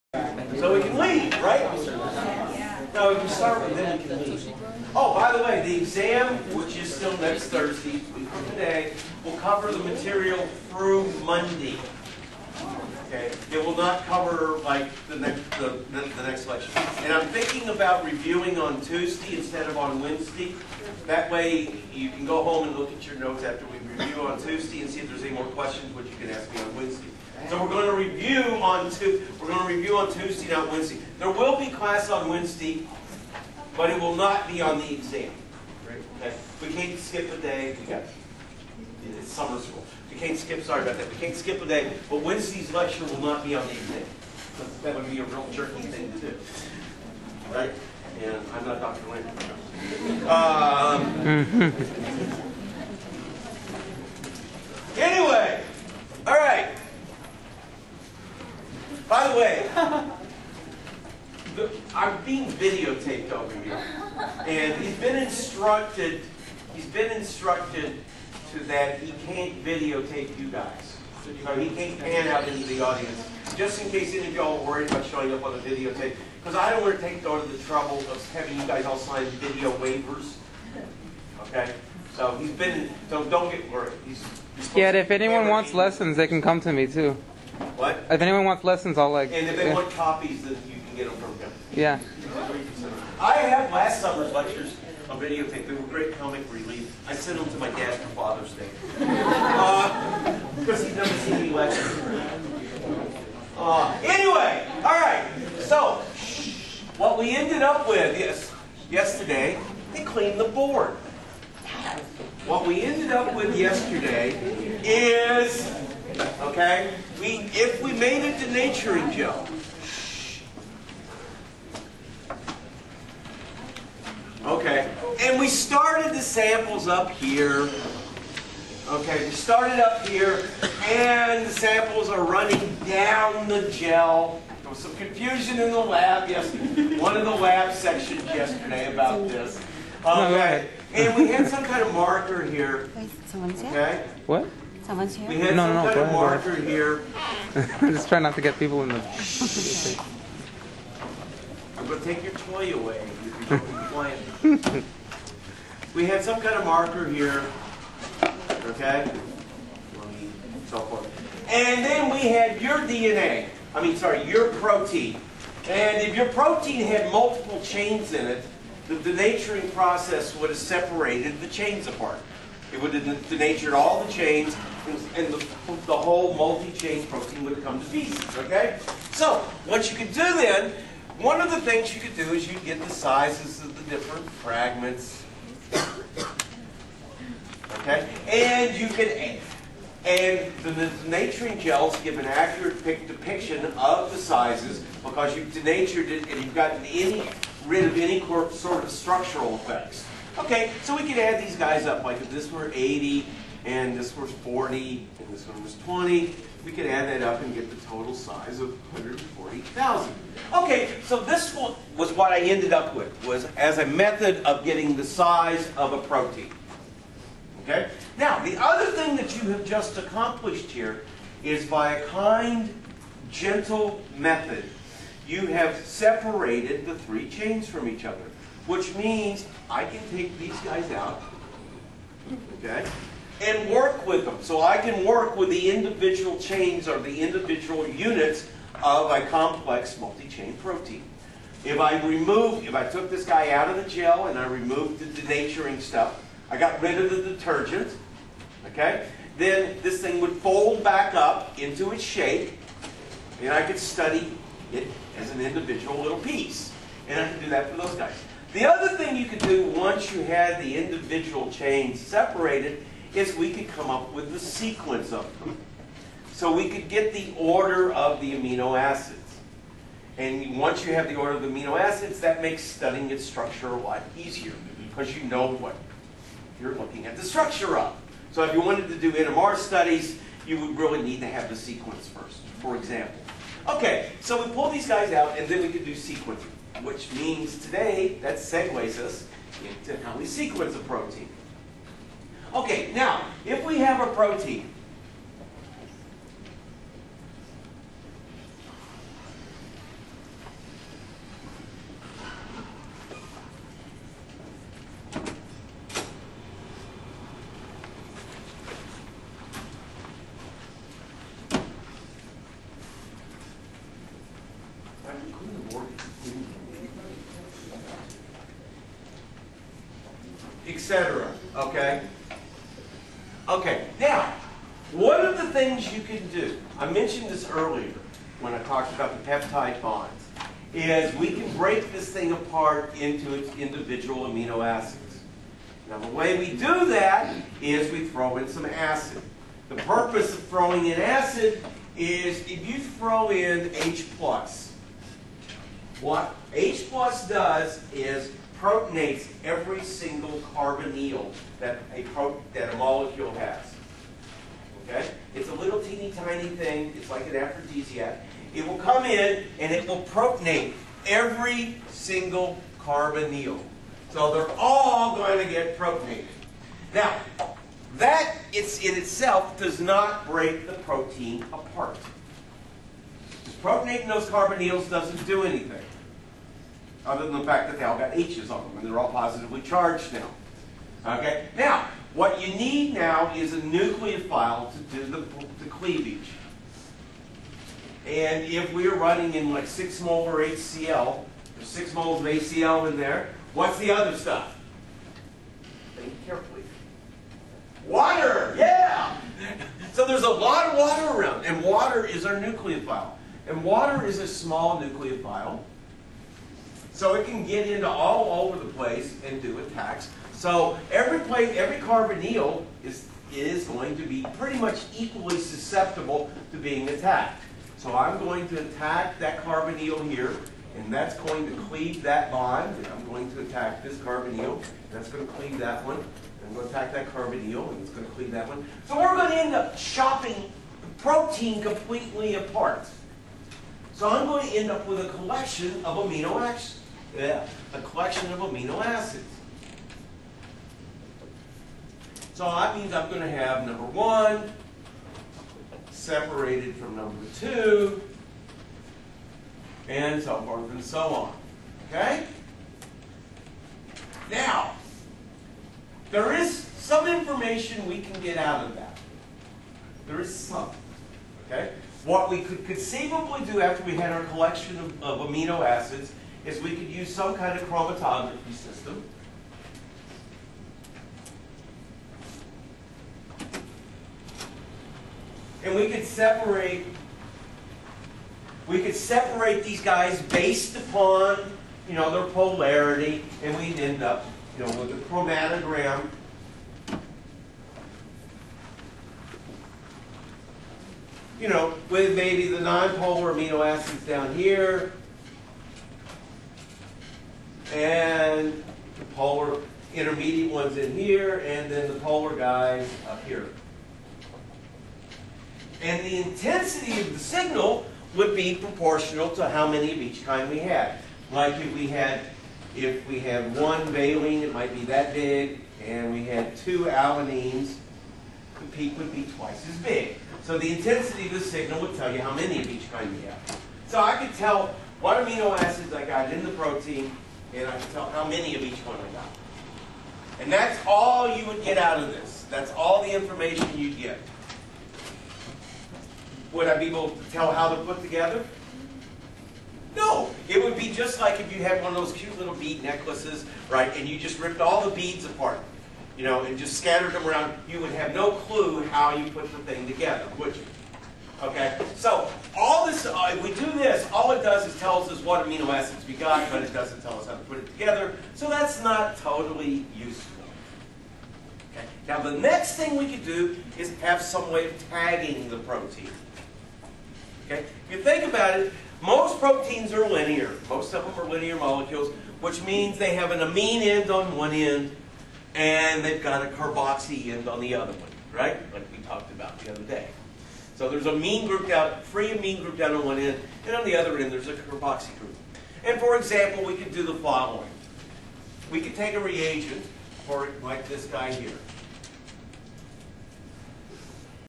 Protein Sequencing (Part 1).wmv
Lecture on Protein Sequencing from Florida International Univercity